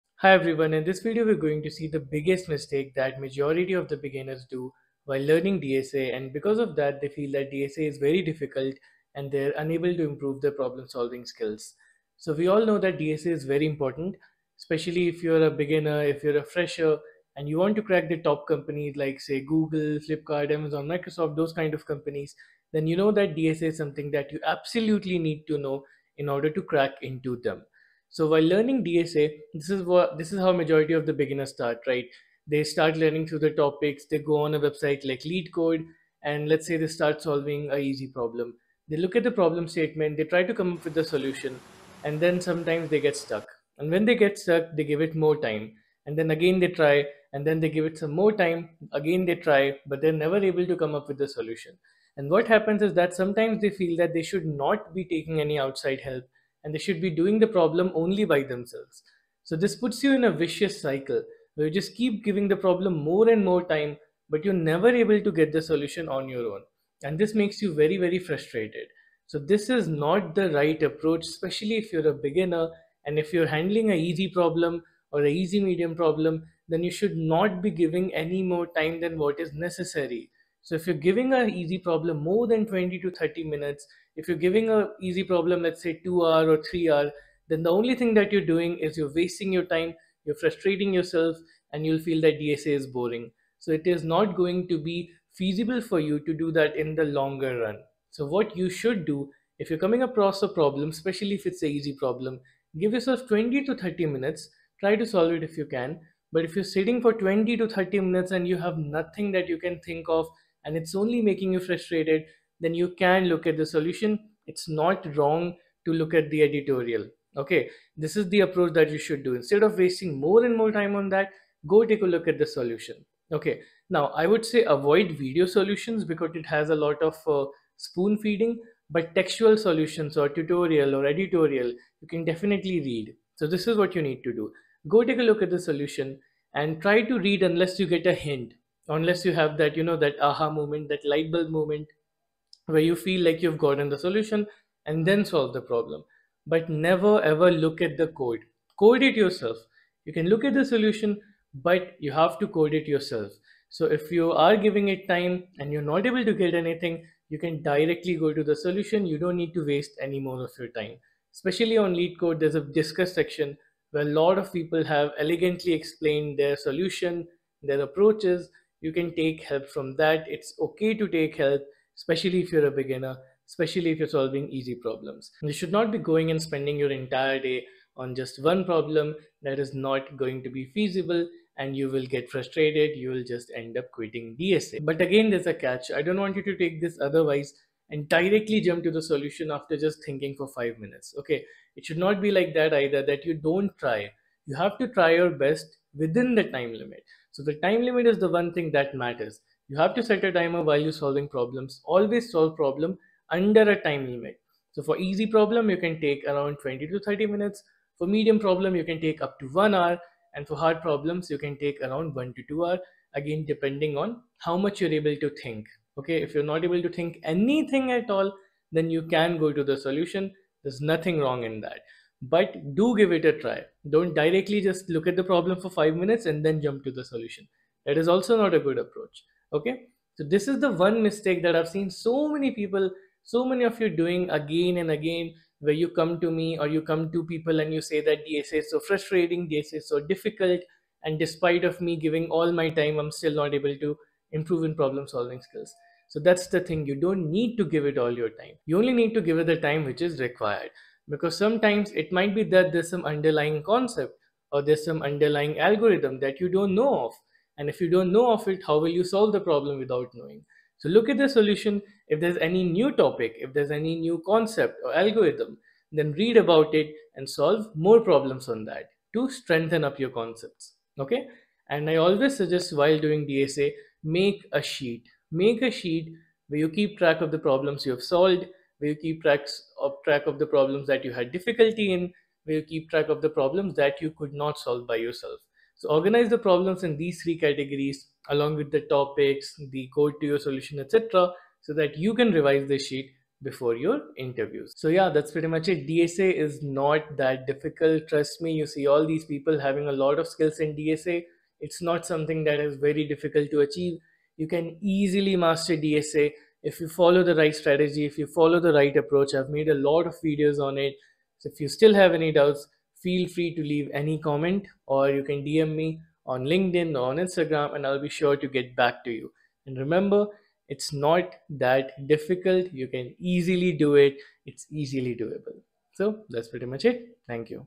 DSA is Difficult If You Make This MISTAKE | Beginner Data Structures AND Algorithms
Hi Everyone,
In this video I have explained why many people get stuck in DSA or are unable to improve in DSA and end up getting demotivated, follow this approach and I guarantee you that you will be able to start improving your problem-solving skills quickly.